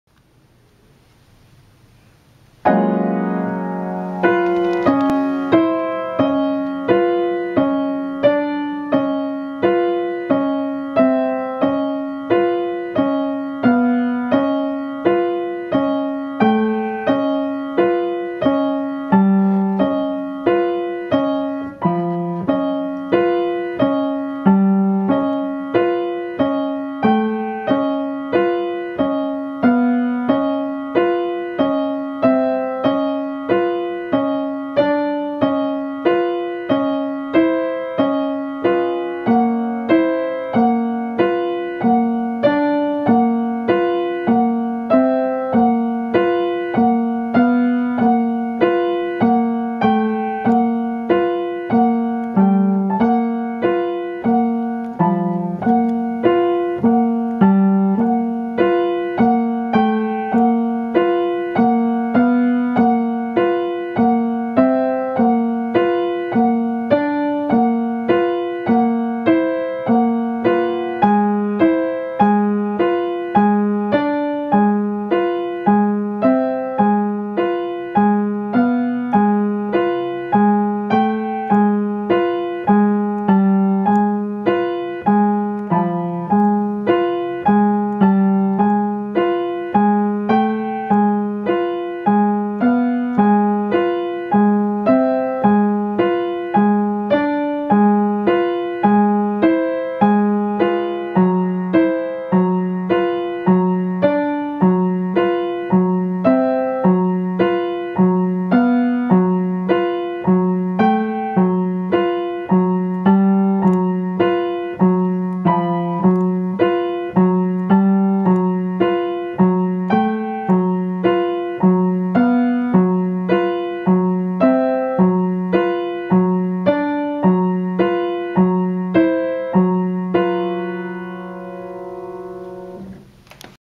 Exercise 2: Thumbs passing under sustained tones.
The Piano School's intern during January 2013 demonstrates technical exercises her teacher used with her in Tokyo.